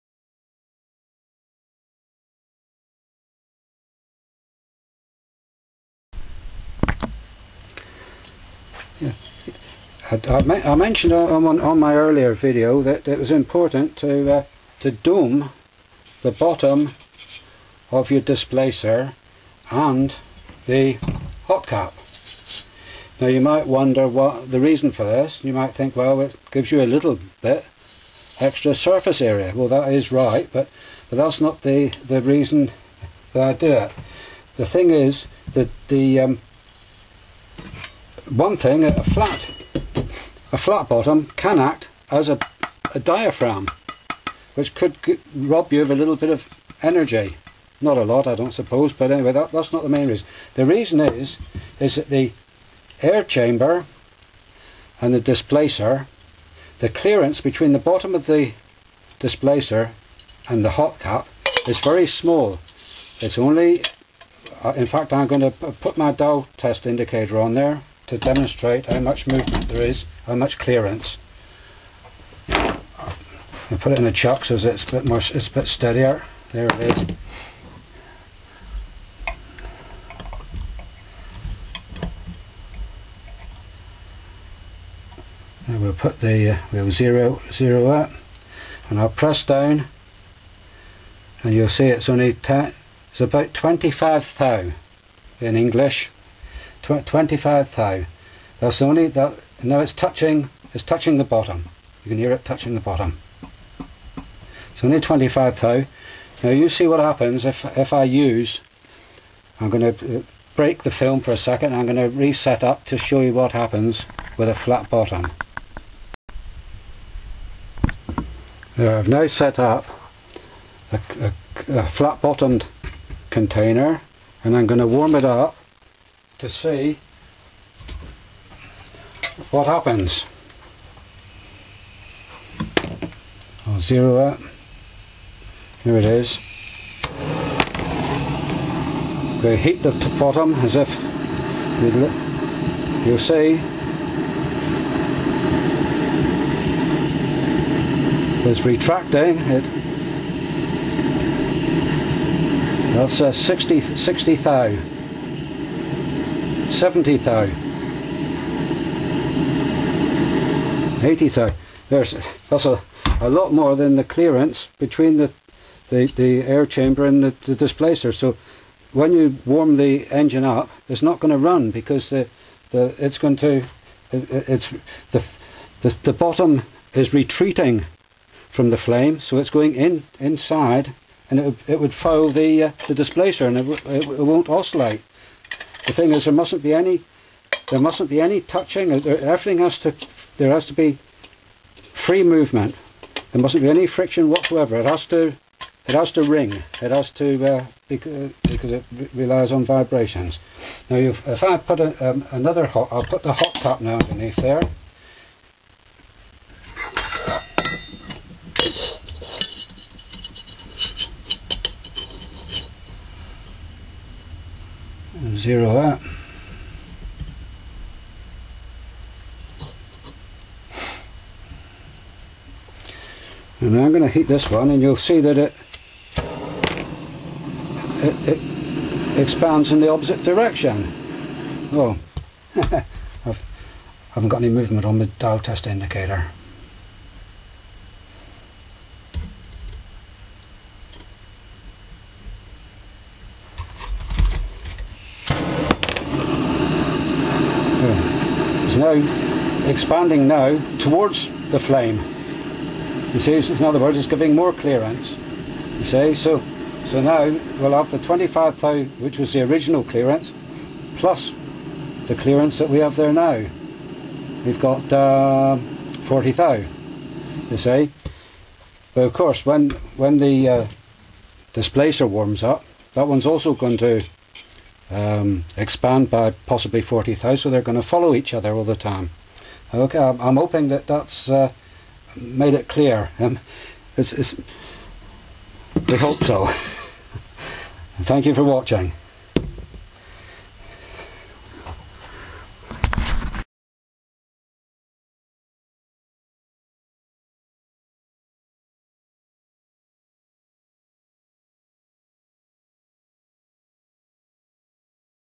Stirling Cycle TMG. Part Five
An explanation of the reason for doming the Hot cap and Displacer.